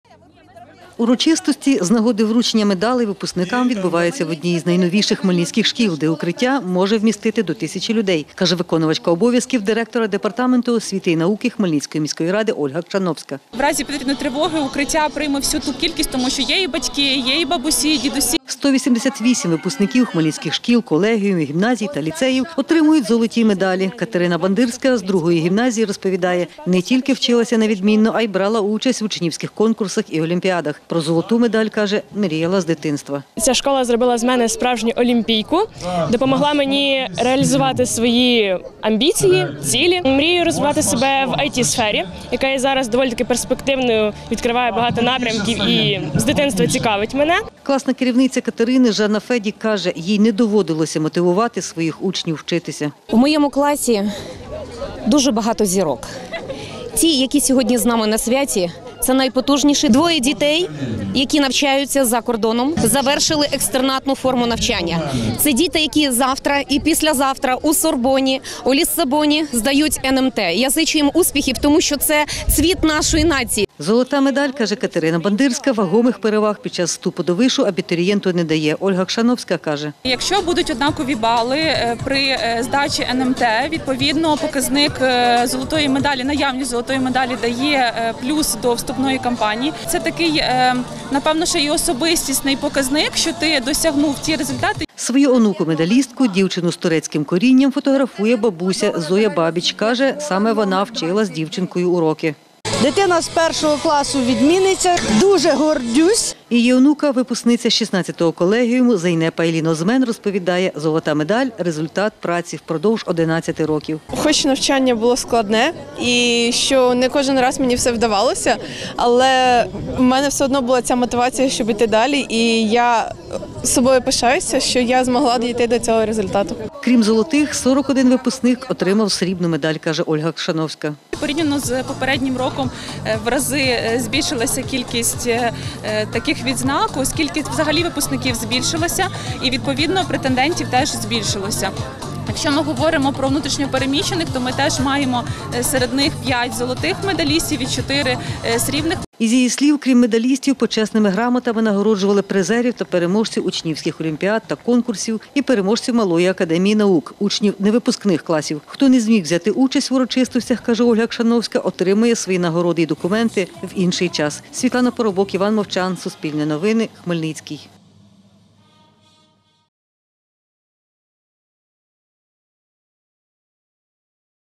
Хмельницькі випускники отримали золоті та срібні медалі
Випускники хмельницьких шкіл, які відмінно навчалися та були призерами і переможцями учнівських олімпіад і Малої академії наук, отримали золоті й срібні медалі та почесні грамоти. Виконувачка обов'язків директора департаменту освіти і науки Хмельницької міської ради Ольга Кшановська розповідає: порівняно з попереднім роком, кількість випускників і кількість медалістів збільшилася.